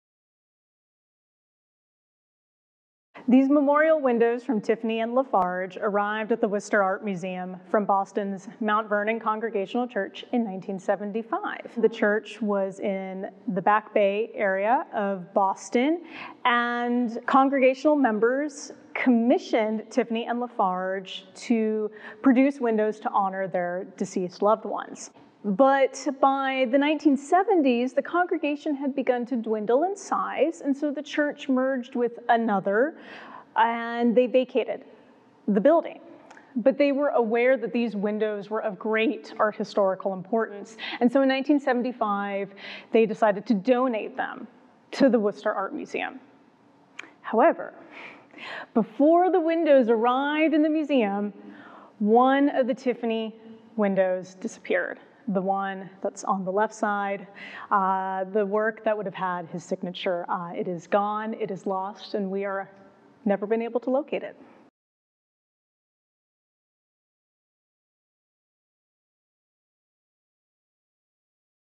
Stained Glass by Tiffany and La Farge: Acquisition
Erin Corrales-Diaz, Assistant Curator of American Art at Worcester Art Museum, talks about WAM’s acquisition of the stained-glass memorial windows by Louis C. Tiffany and John La Farge. Originally commissioned for Boston's Mount Vernon Congregational Church in the late 1890's, the works were donated to WAM in 1975 when the church vacated its building. The windows are the focus of the WAM Exhibition “Radiance Rediscovered: Stained Glass by Tiffany and La Farge”.